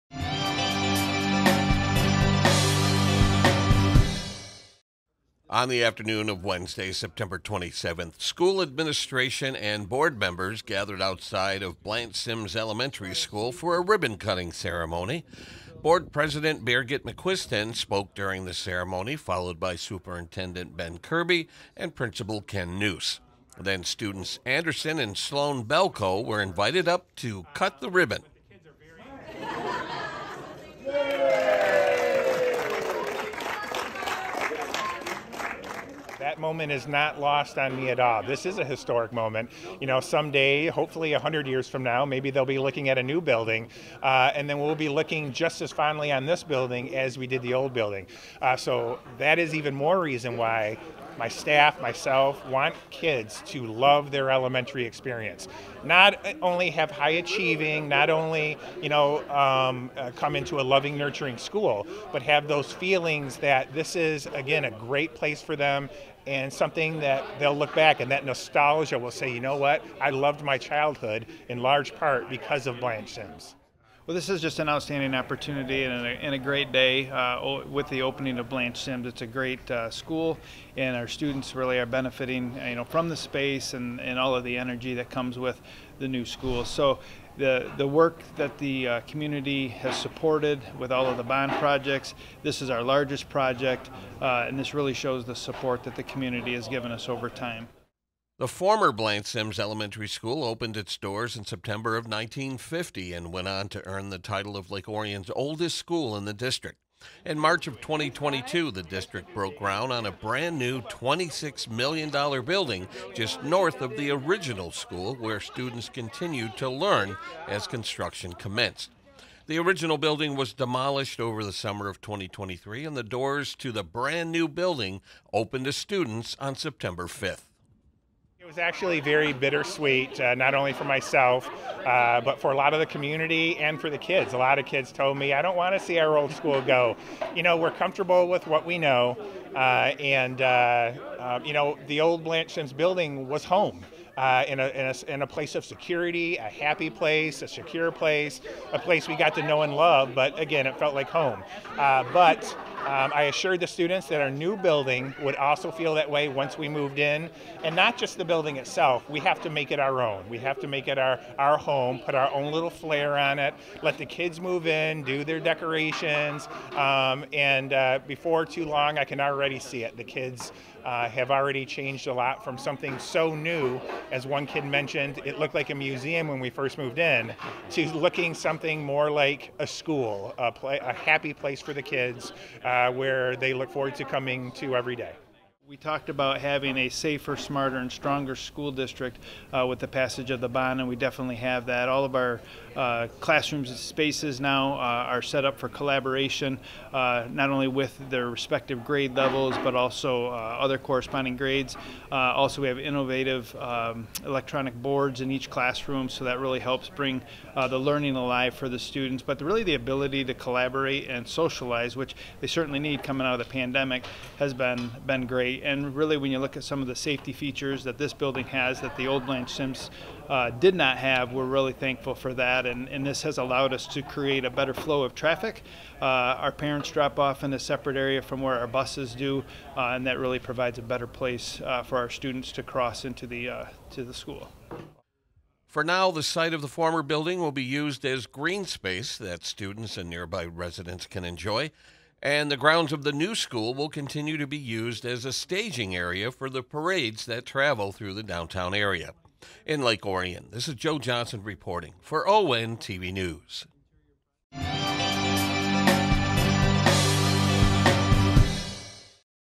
ONTV News Break: Blanche Sims Elementary School Grand Opening
On Wednesday, September 27, 2023, LOCS administration and school board members gathered at Blanche Sims Elementary School for a ribbon-cutting ceremony.